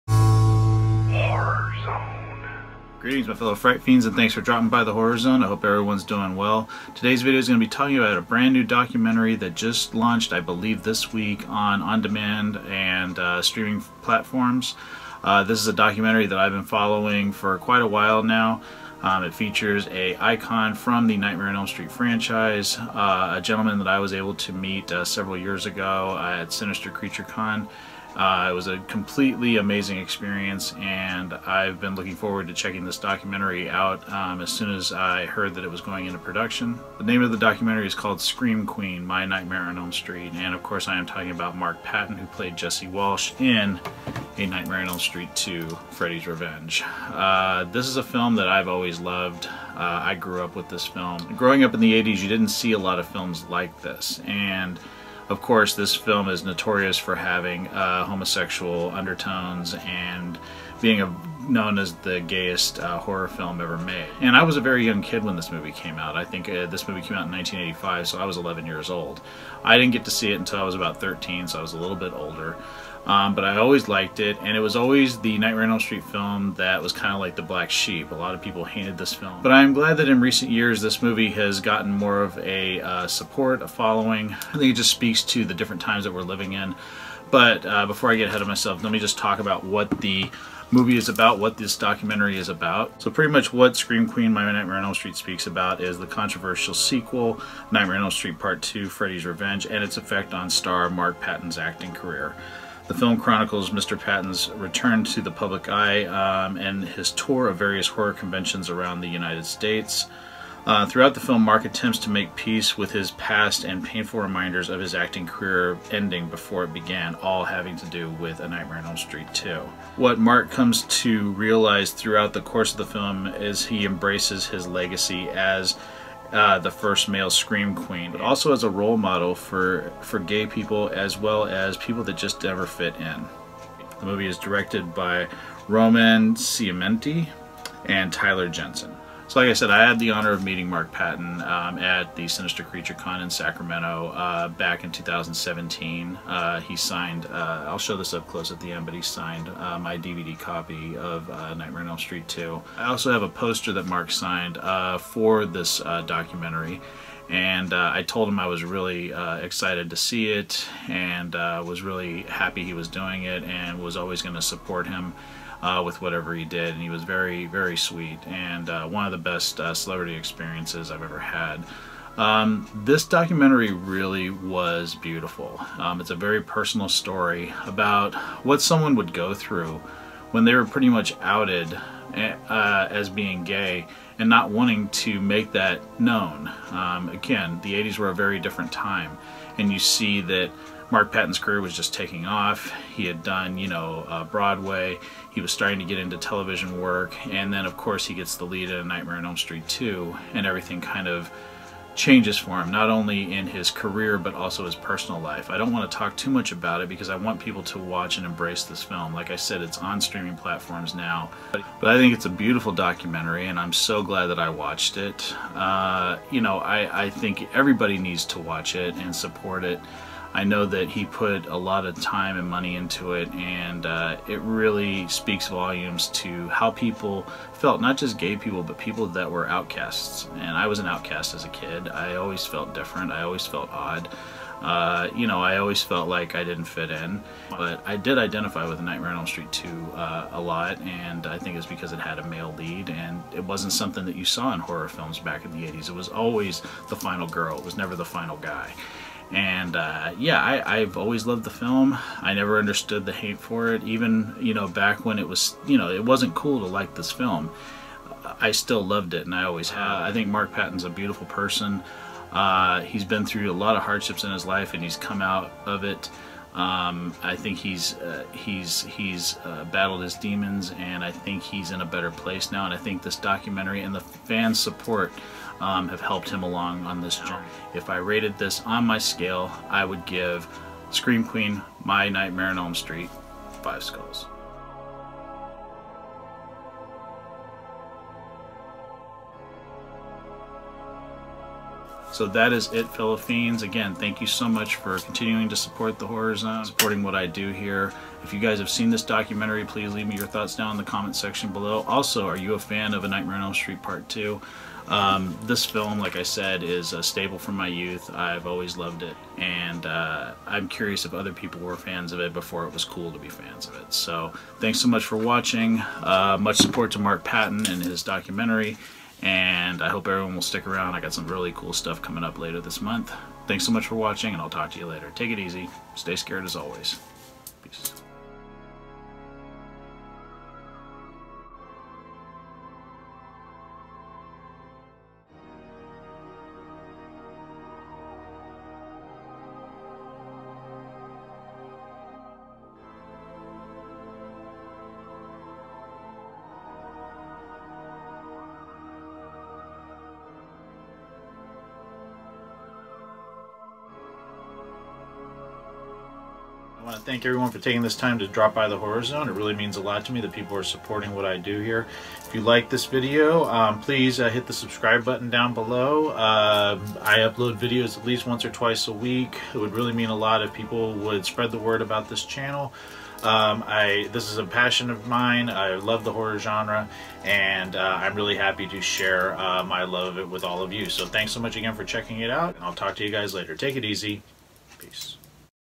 Horror Documentary Review: Scream, Queen! My Nightmare on Elm Street
My review of the Horror Documentary "Scream, Queen! My Nightmare on Elm Street"